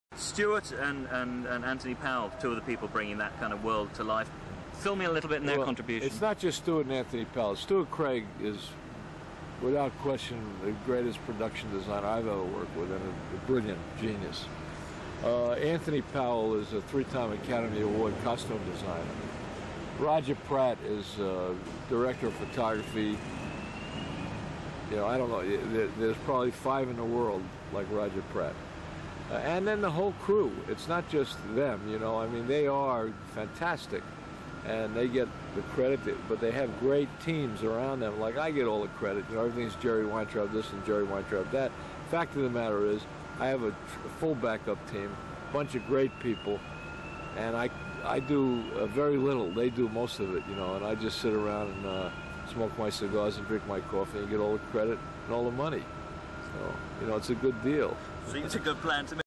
Jeff Weintraub on his Crew, 1990's - Film 93176
Interview with film producer Jerry Weintraub about 'The Avengers' (1998). Jeff is asked about Stuart Craig and Anthony Powell. He complements them and makes sure to say how all the crew contribute towards the film. He says how he smokes cigars while everyone else does the work. That does sound like a good deal!